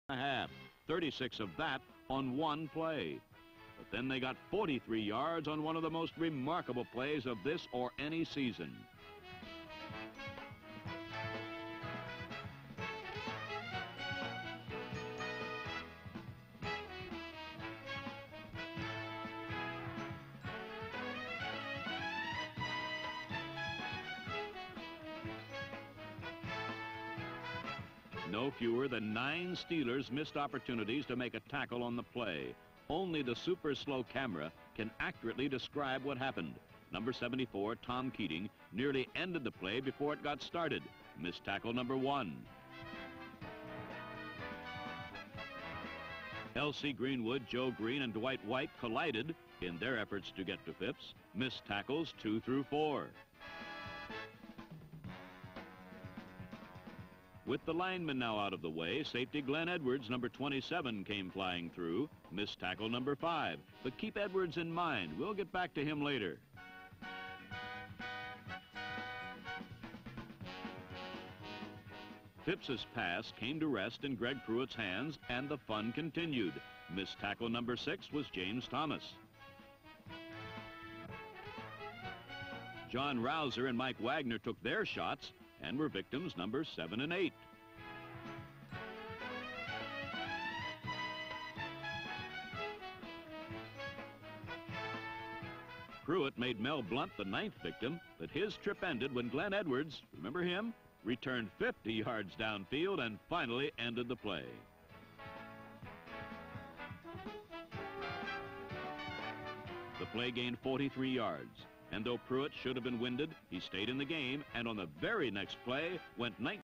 Amazing Sack Avoid + Catch & Run By Phipps & Pruitt
1973 Browns vs Steelers